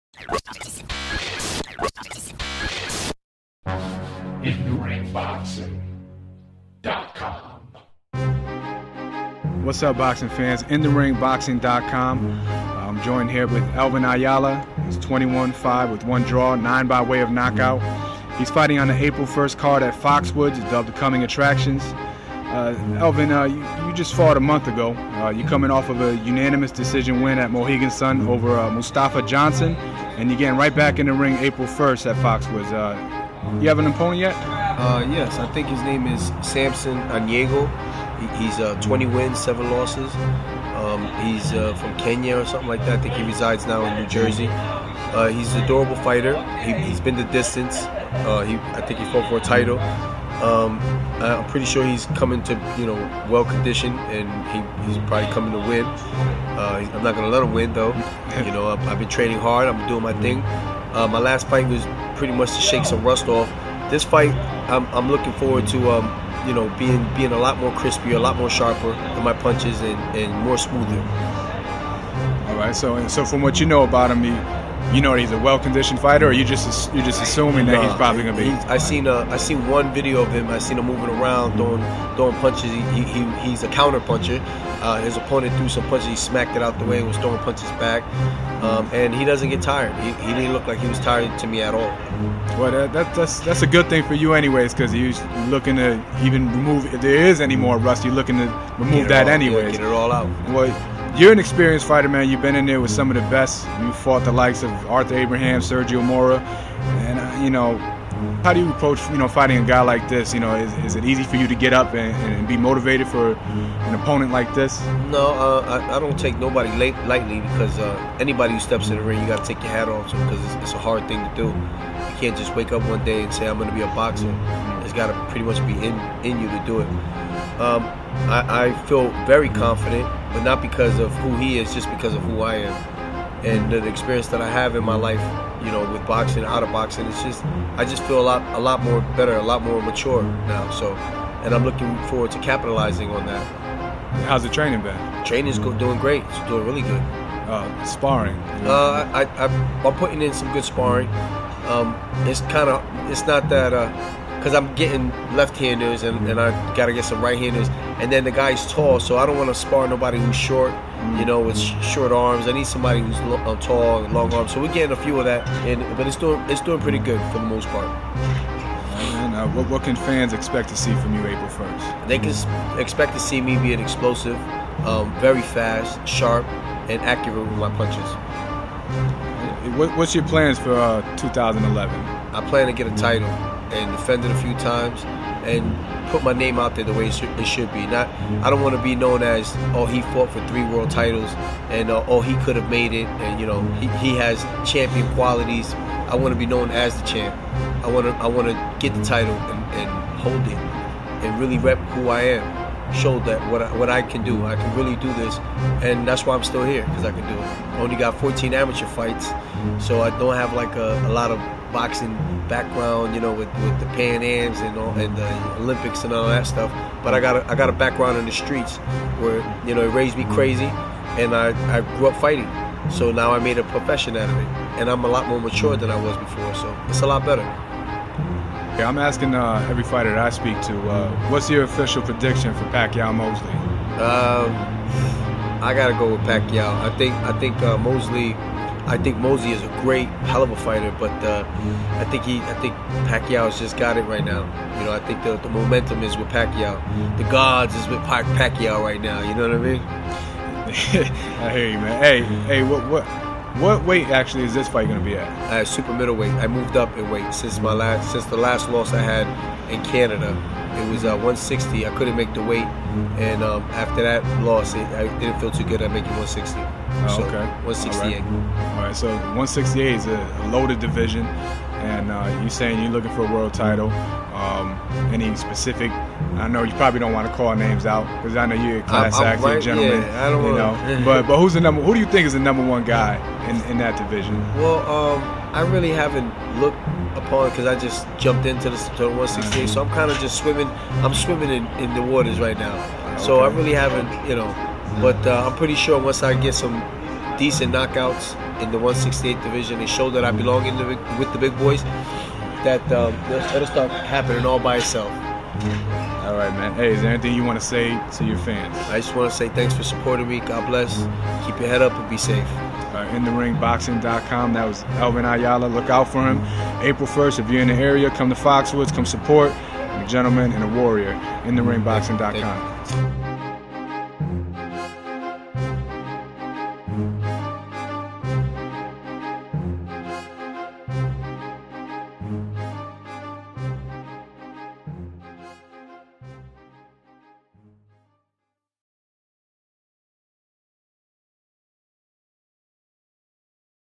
Elivin Ayala Interview - intheringboxing.com
Carlos Cruz from intheringboxing.com interviewed Super Middleweight Elvin Ayala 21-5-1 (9 ko's) as he prepares for his April 1st Showdown against Samson Onyango 20-7 (13 ko's).